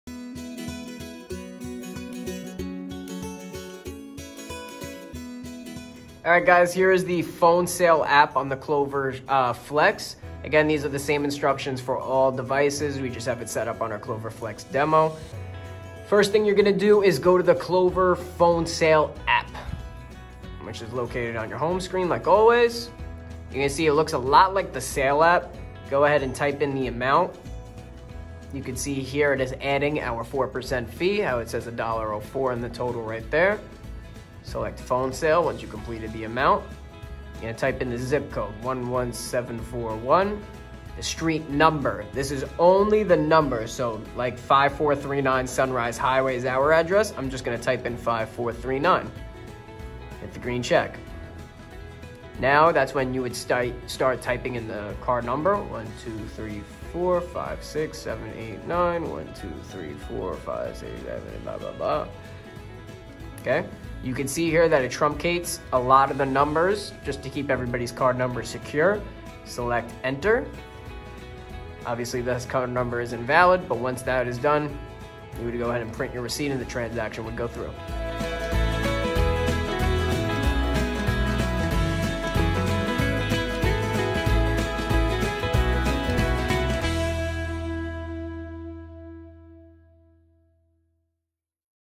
Clover POS - How to do an over the phone sale on the Clover Flex
In todays video, the UpNexa tech team shows how to complete an over the phone sale on the Clover Flex. This is the same instructions for the Clover Mini.